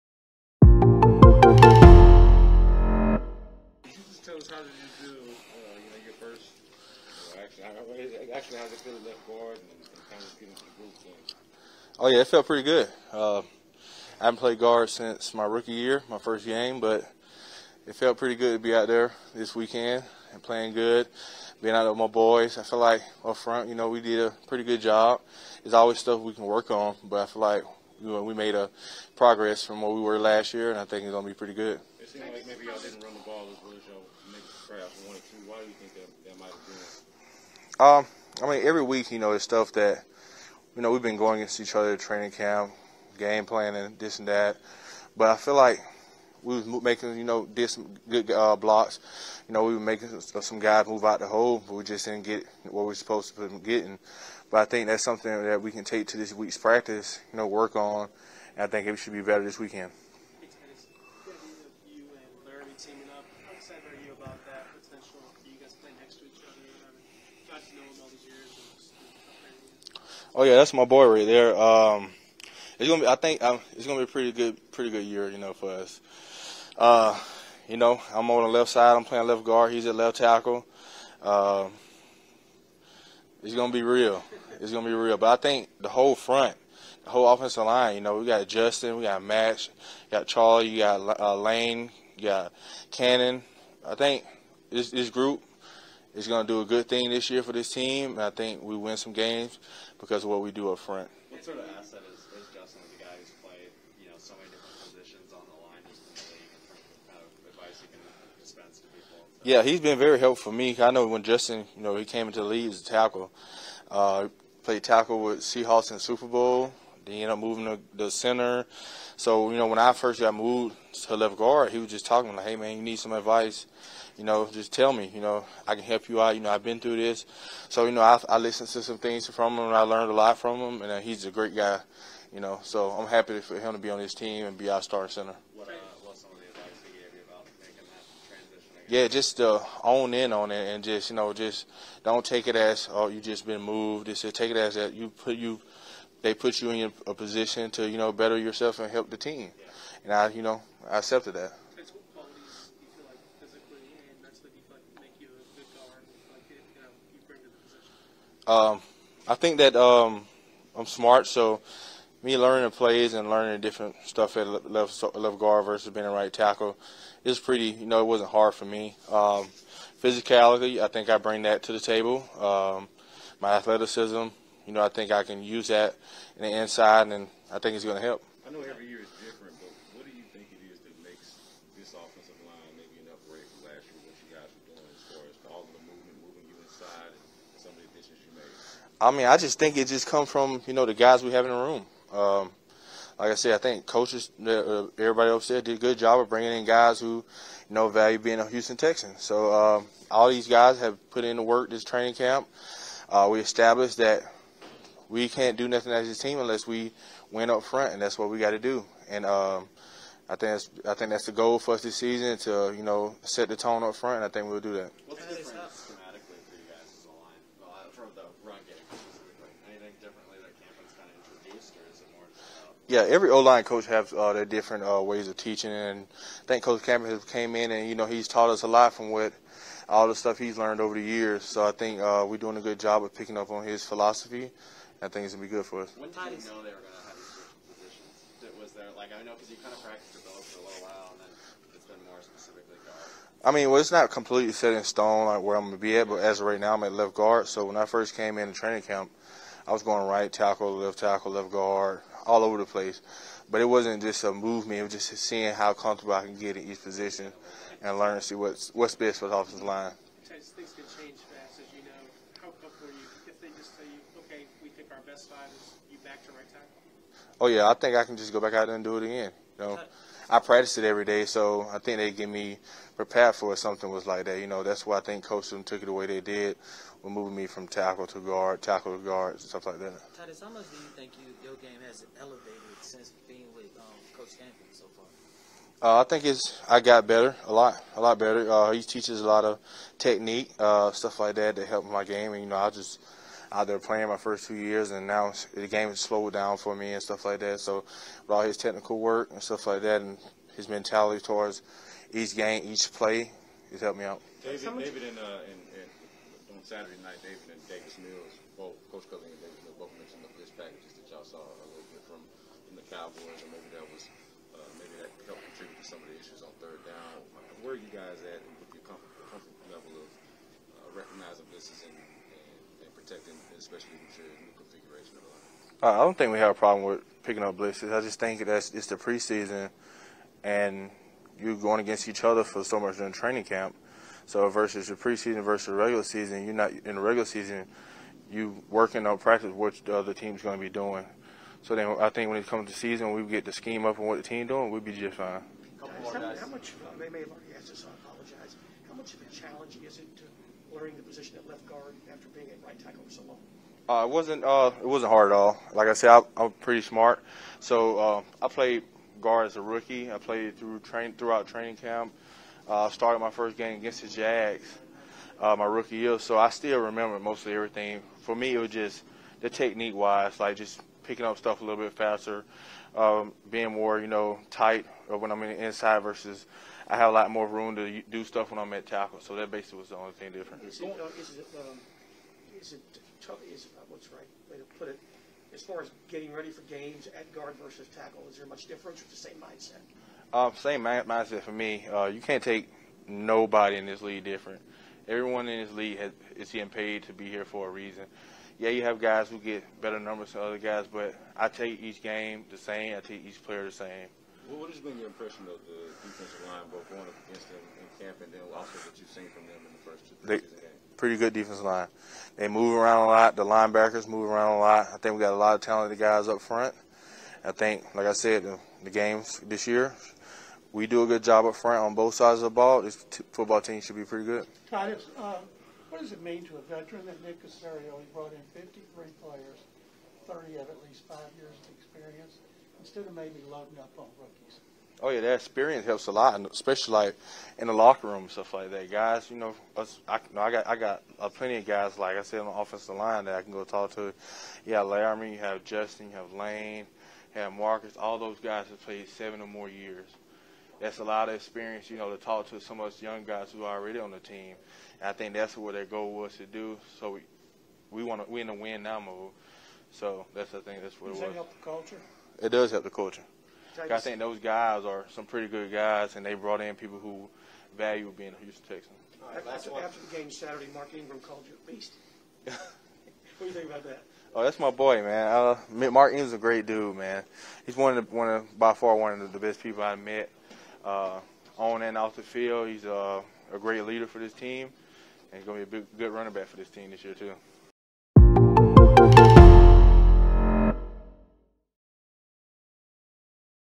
Texans OL Tytus Howard talks transitioning from right tackle to left guard
Tytus Howard was clear earlier in training camp when he spoke with In The Loop about his goal to win the job at right tackle.

But that job's likely going to second-year tackle Charlie Heck, setting up an interesting tandem on the left side with Howard and Pro Bowl left tackle Laremy Tunsil.